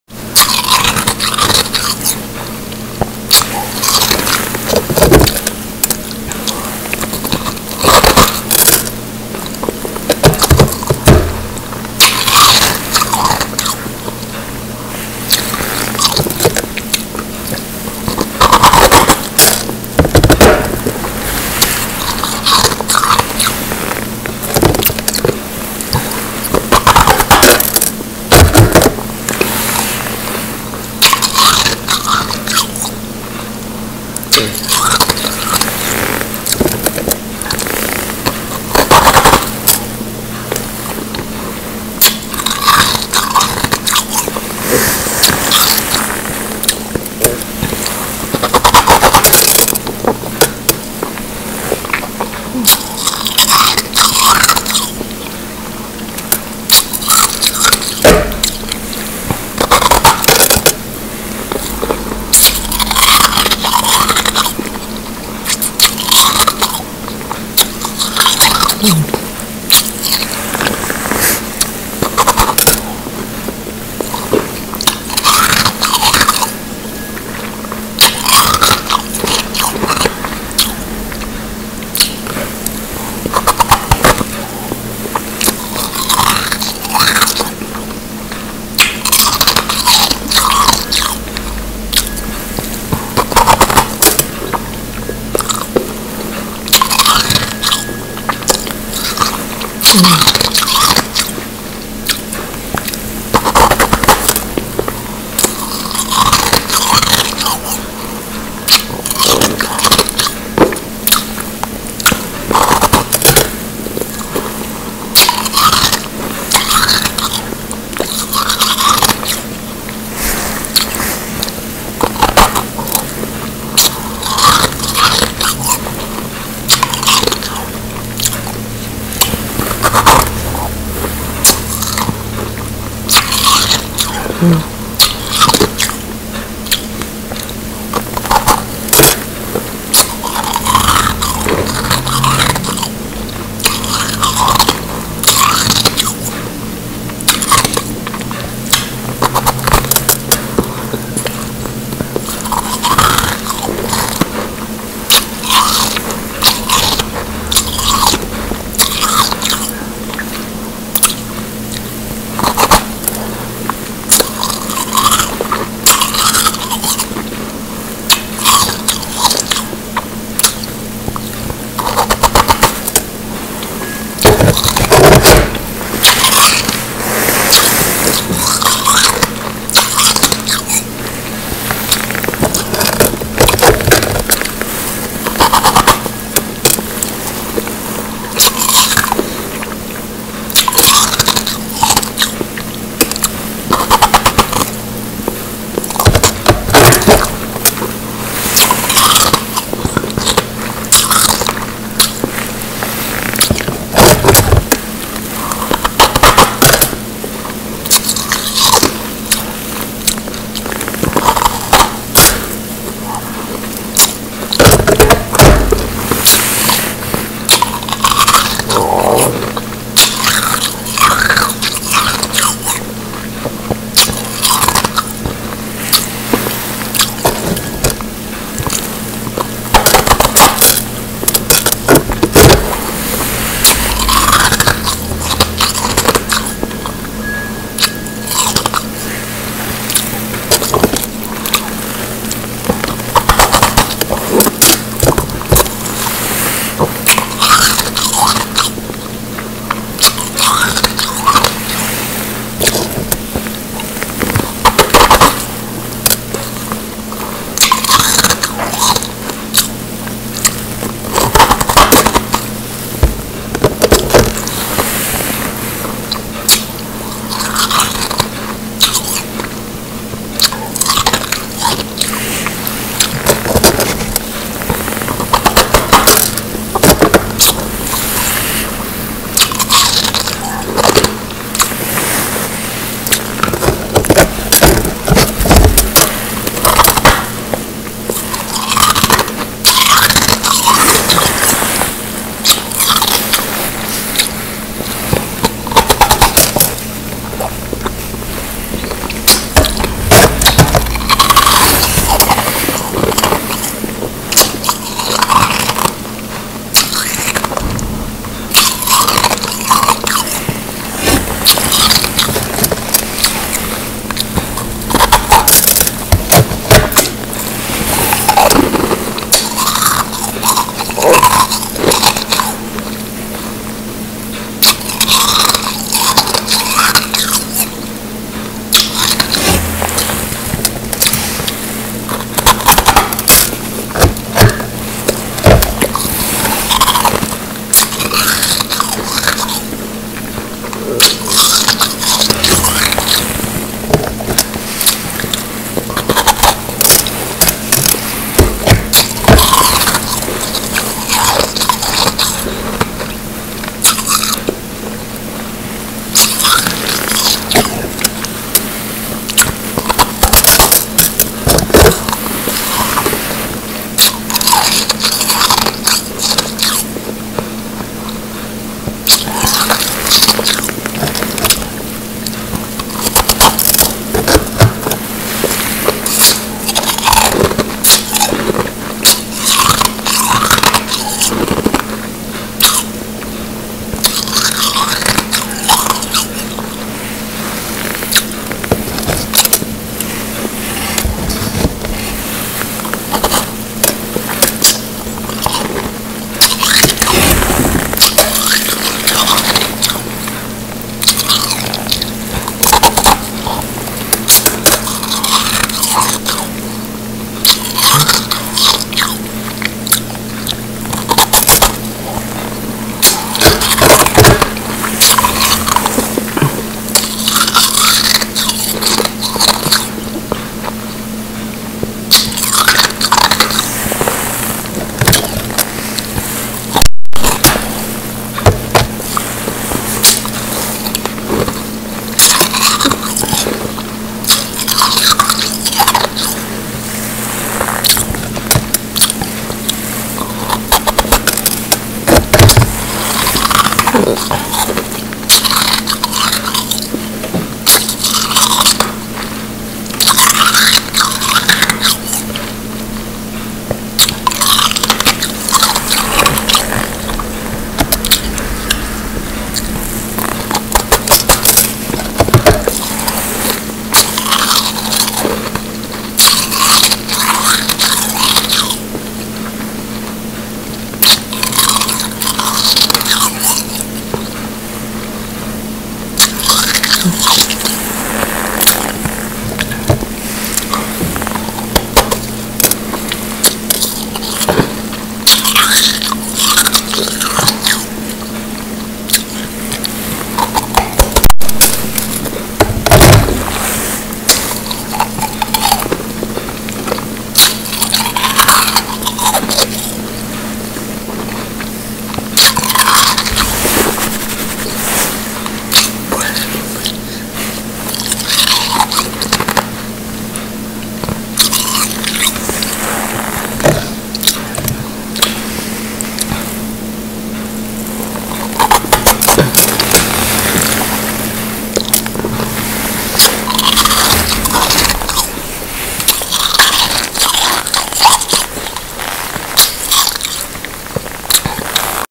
Ice eating ASMR | Slow eating | Snow eating | Ice Snow Life | ASMR Ice is Life
Hi everyone! Since I eat super slow I thought why not promote the Slow Ice Eating since you can really hear the crunches in detail 😊@ASMRIceIsLifer